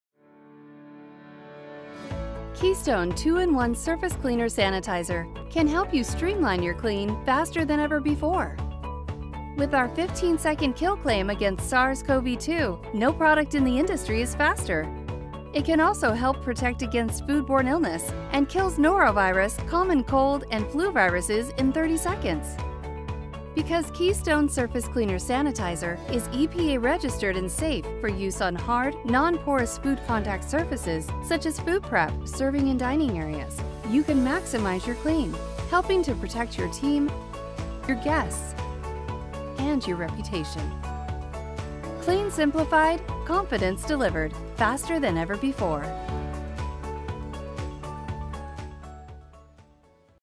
Keystone Surface Cleaner Sanitizer RTU
EPA-registered 2-in-1 RTU cleaner and sanitizer requires only two steps to clean and sanitize simplifying how you clean food contact surfaces. Kills SARS-CoV-2 in 15 seconds1. Kills Norovirus, common cold and flu viruses in 30 seconds2. EPA No. 1677-259.